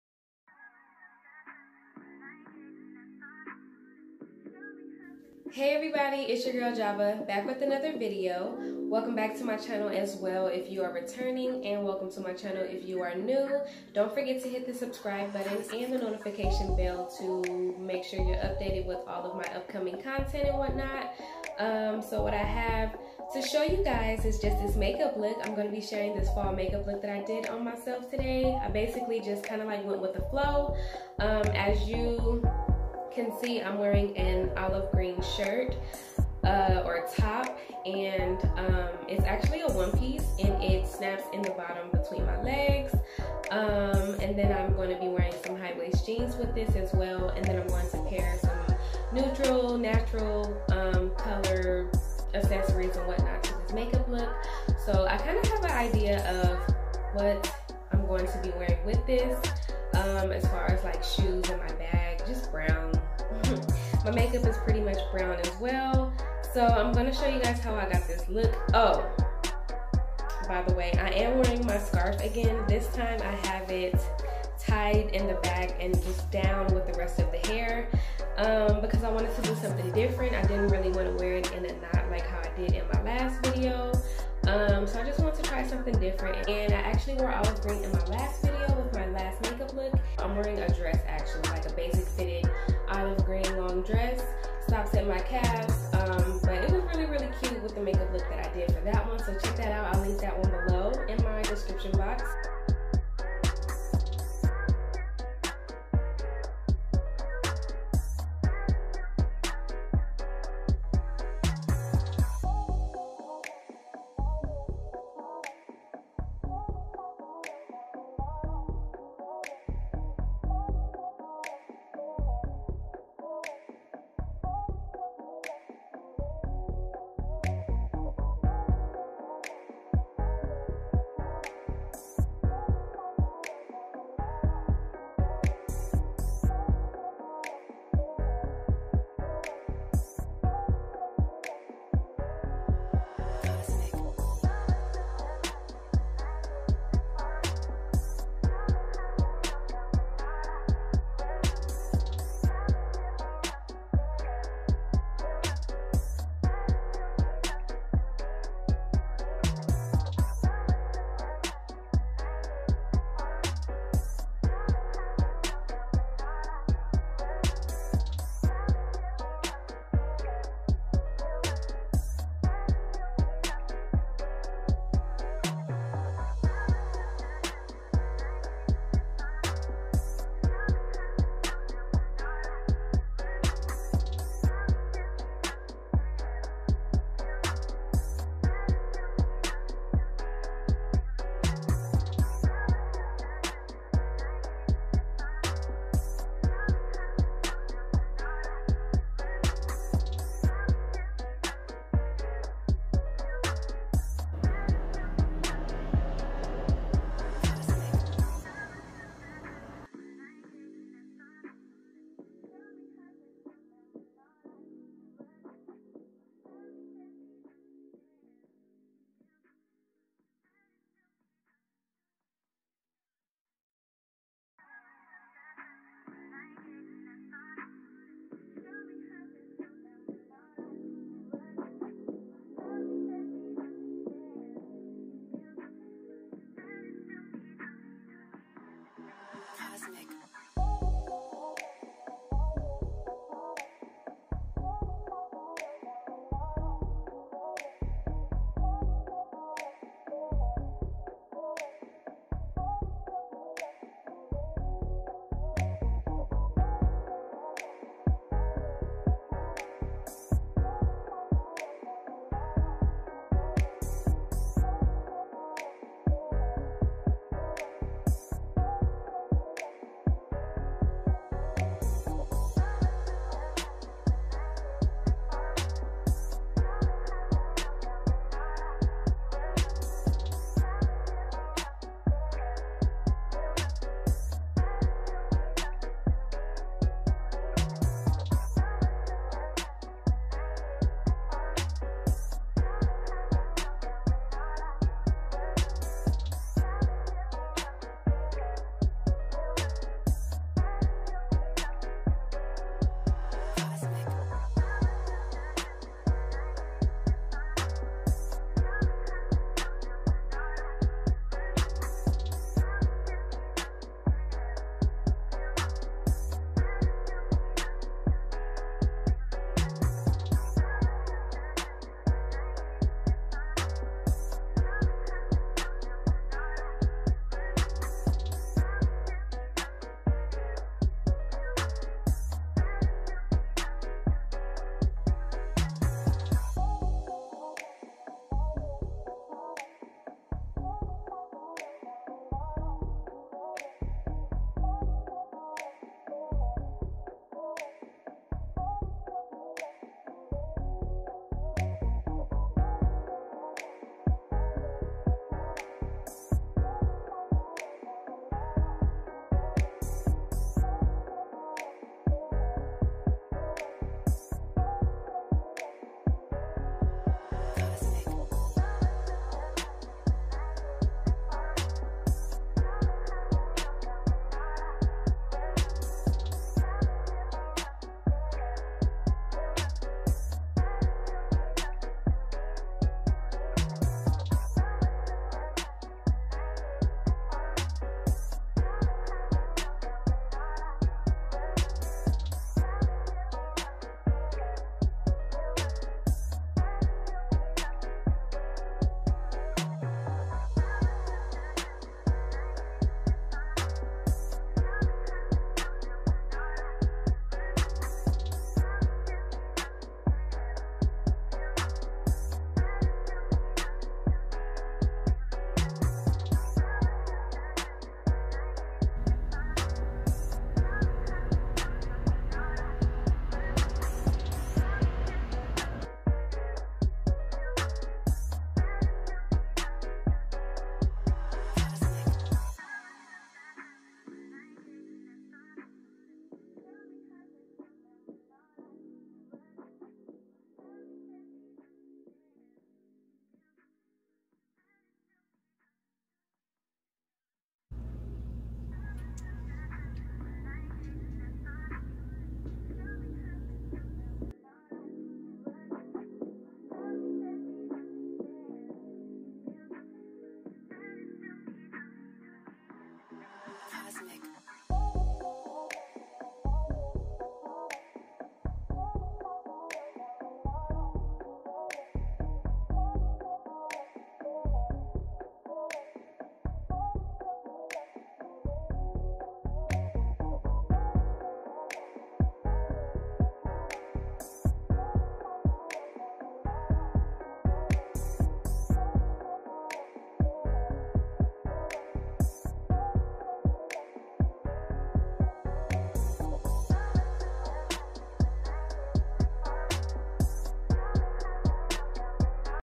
Fall GRWM: Soft Chocolate Nude Makeup Glam|Java Juice
Hey Girl, Hey! Here's another Fall Szn Makeup Look. This Chocolate Nude look is definitely a cute makeup look for WOC. Any questions about this look??. I'll answer in the comments below.

•COMMENT if you love this look!